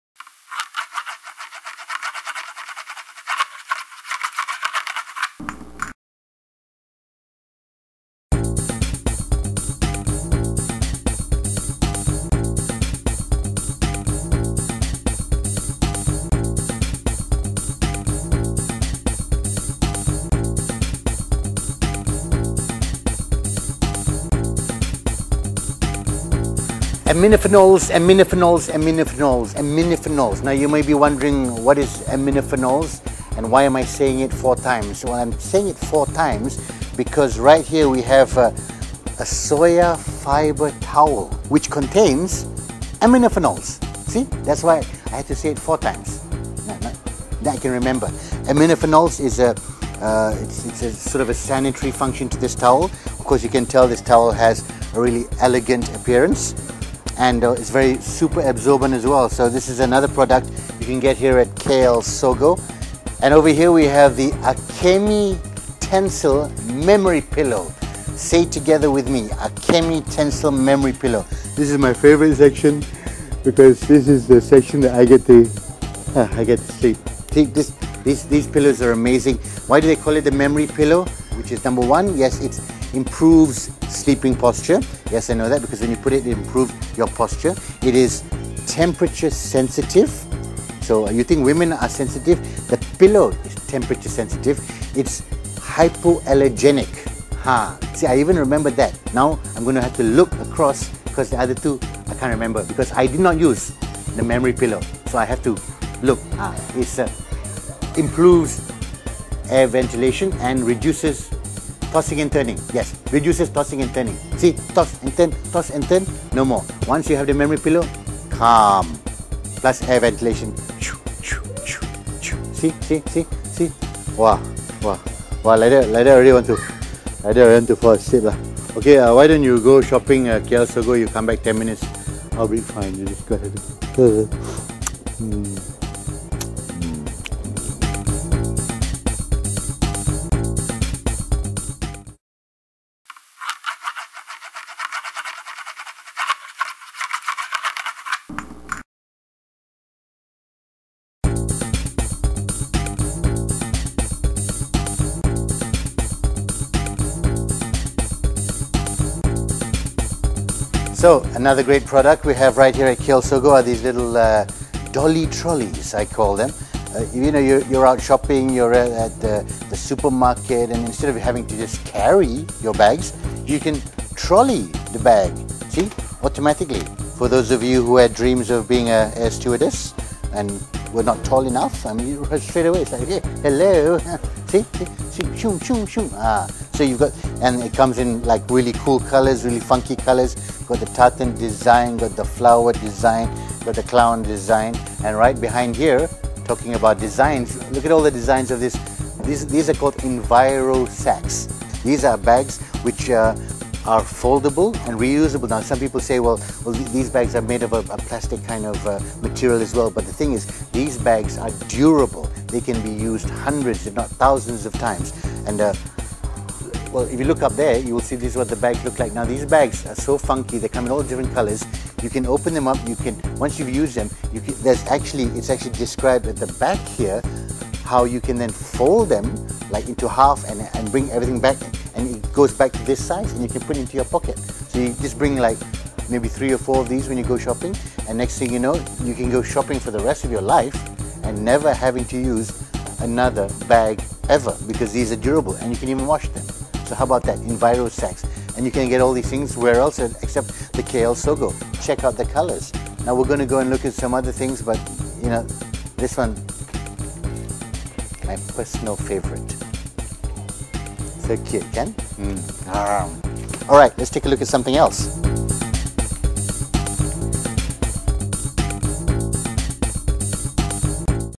Creature Comforts and Shopping Buddies
Soya Fiber Towels for that warm and fuzzy after shower feeling, Akemi Tencel Memory Pillow for a good night's rest, and according to Harith, if you're an aspiring air stewardess, you can always shop with the funky trolley bags!